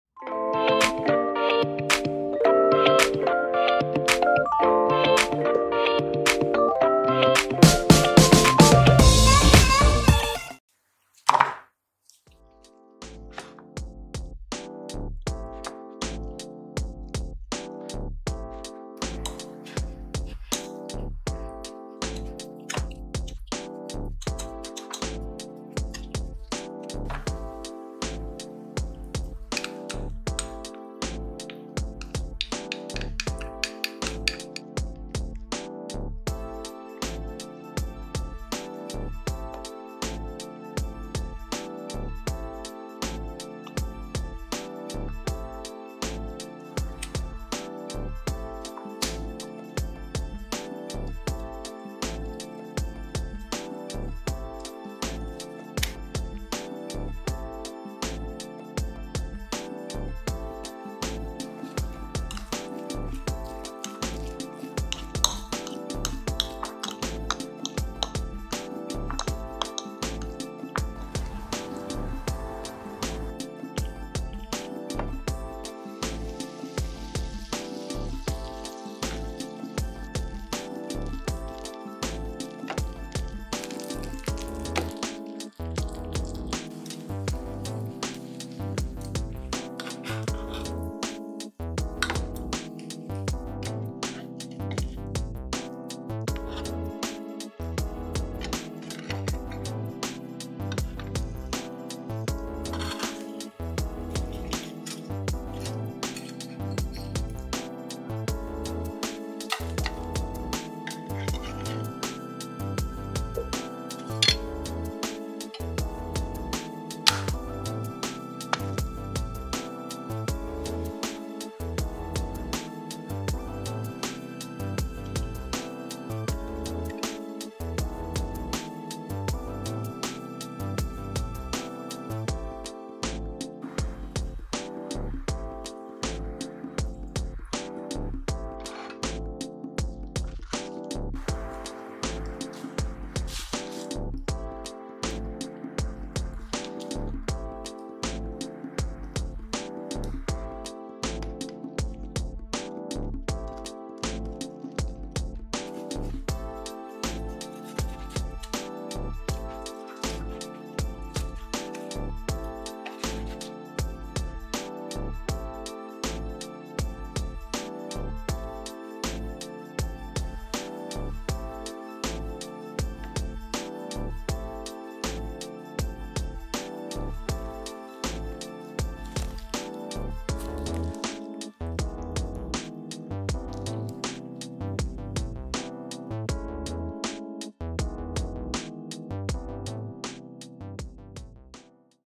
Try this recipe 😍 so YUMMY ! Roast beef egg toast | Karmel taste
Healthy, Healthy  and Delicious Breakfast . If you have eggs and bread, you will be addicted 😍😍
Ingredients:
Roast beef slices.
Boiled eggs.
Cream cheese.
Mayonnaise.
Yellow mustered.
Chili flakes.